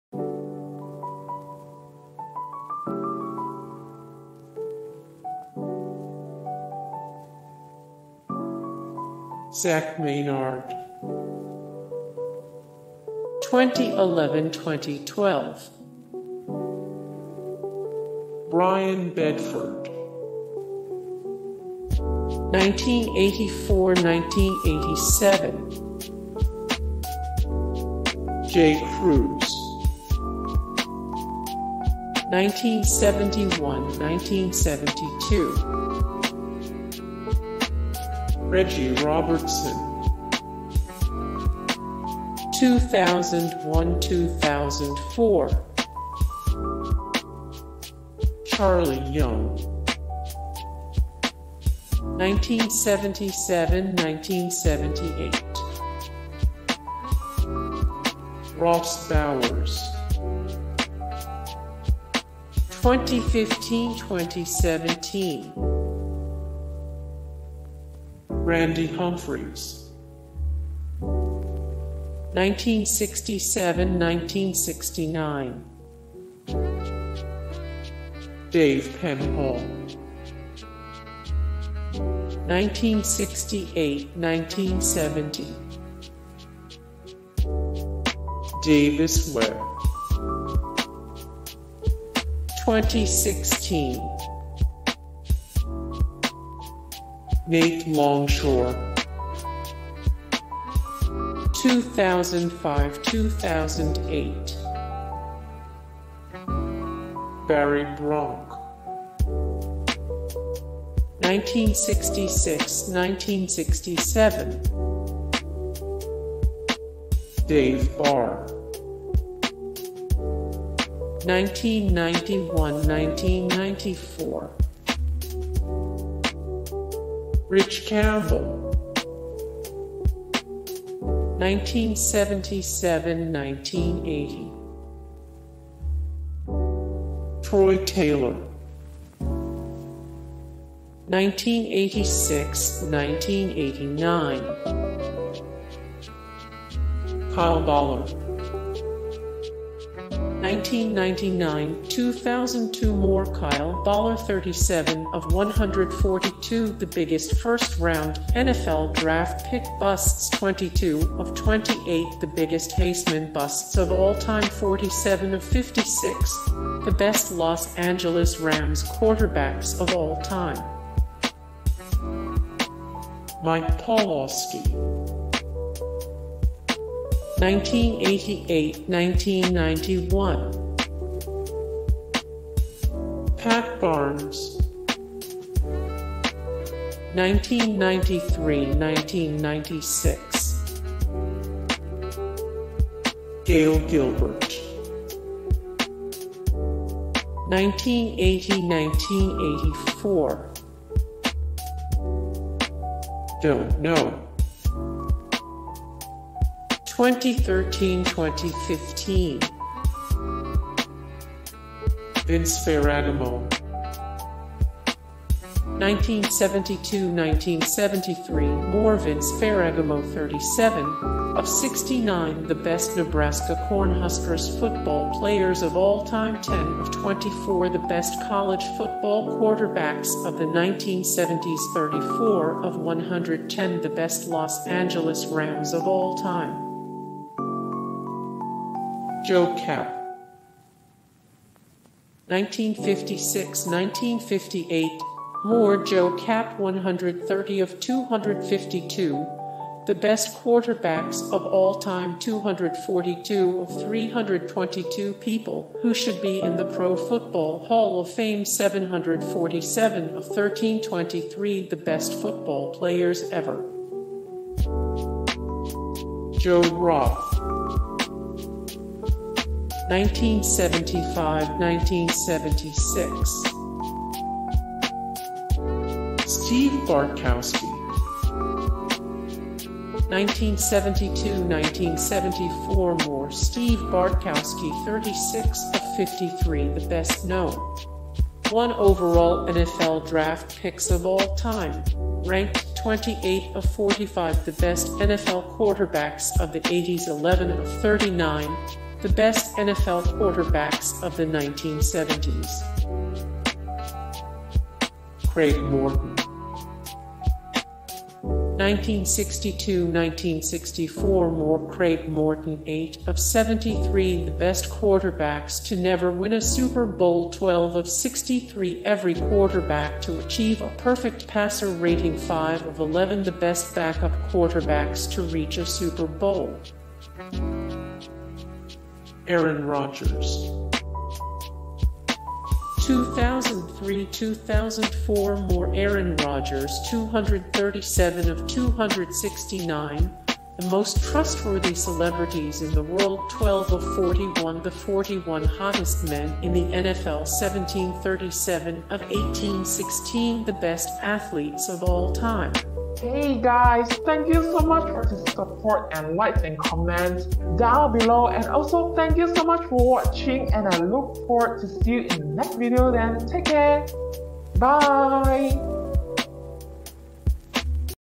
The Best California Golden Bears Quarterbacks Of All Time 💟
College football fans, were ranking the best California Golden Bears quarterbacks of all time. It includes every starting quarterbackthroughout California Golden Bears history, including current quarterbacks and past ones. This California football roster has famous All-Americans, California quarterbacks in the NFL, and Golden Bears players in the Pro Football Hall of Fame. Featuring some of the greatest college quarterbacks of all time, the best California QBs include Jared Goff, Aaron Rodgers, David Webb, and Troy Taylor.In the 2017 FBS season, the current California Golden Bears QBs are Ross Bowers, Chase Forrest, and Brandon McIlwain.Who are the greatest quarterbacks in California Golden Bears history? They're all on this comprehensive list of University of California, Berkeley football players who have played as quarterback. more
0:00 - Intro
0:00:08 - Zach Maynard
0:00:18 - Brian Bedford
0:00:29 - Jay Cruze
0:00:40 - Reggie Robertson
0:00:51 - Charlie Young
0:01:02 - Ross Bowers
0:01:13 - Randy Humphries
0:01:24 - Dave Penhall
0:01:35 - Davis Webb
0:01:45 - Nate Longshore
0:01:56 - Barry Bronk
0:02:07 - Dave Barr
0:02:18 - Rich Campbell
0:02:29 - Troy Taylor
0:02:40 - Kyle Boller
0:03:07 - Mike Pawlawski
0:03:18 - Pat Barnes
0:03:29 - Gale Gilbert
0:03:40 - Don't Know
0:03:50 - Vince Ferragamo
0:04:18 - Joe Kapp
0:04:46 - Joe Roth
0:04:57 - Steve Bartkowski
0:05:27 - Craig Morton
0:05:52 - Aaron Rodgers